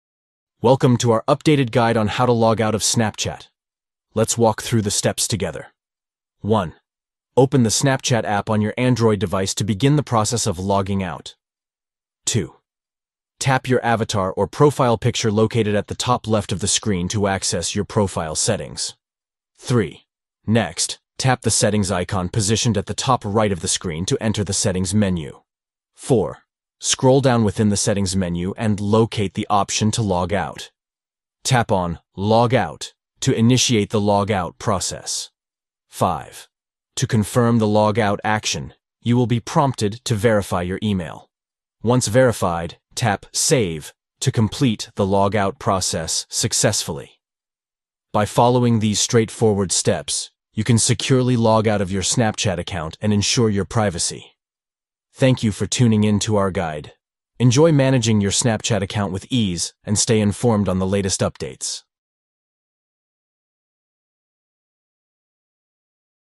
How To Log Out Of Snapchat - Updated
Learn the updated method to log out of your Snapchat account with these simple steps:

1. Open Snapchat app on your Android device.
2. Tap your avatar or profile picture at the top left of the screen.
3. Tap the settings icon at the top right of the screen.
4. Scroll down and tap Log out.
5. Verify your email then tap Save.

Ensure the security and privacy of your Snapchat account by following these straightforward instructions. Watch now and stay informed on how to log out of Snapchat efficiently.